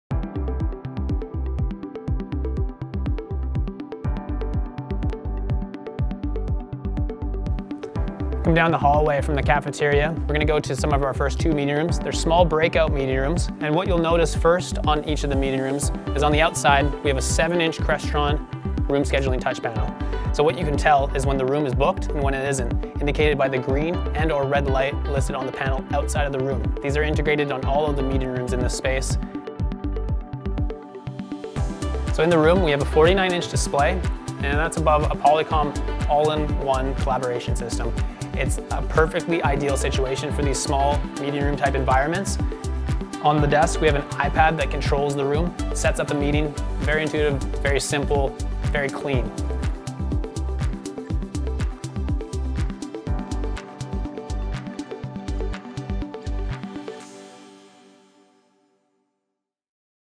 Evolution AV - Breakout Rooms
In this video, Kyle breaks down the setup of a standard collaborative breakout room. Featuring components from Crestron Electronics, Sony, and Polycom all working together to create a seamless collaborative experience.